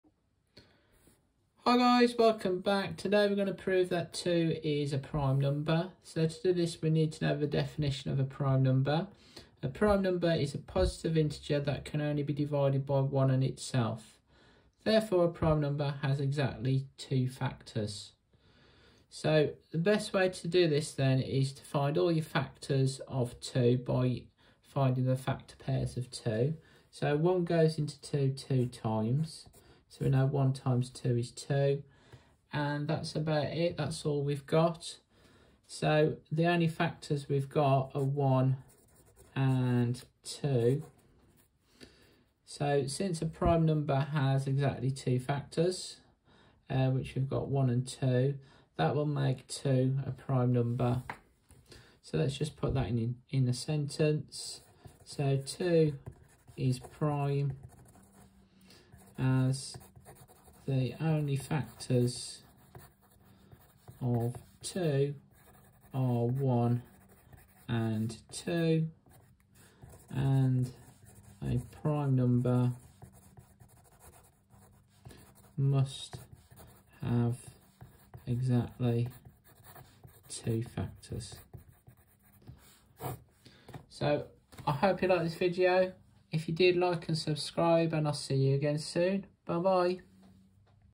Prove That 2 Is A Prime Number
In this video you will learn that 2 is a prime number. A prime number is a number with only 2 factors. Since the only factors of 2 are 1 and 2, this makes 2 a prime number.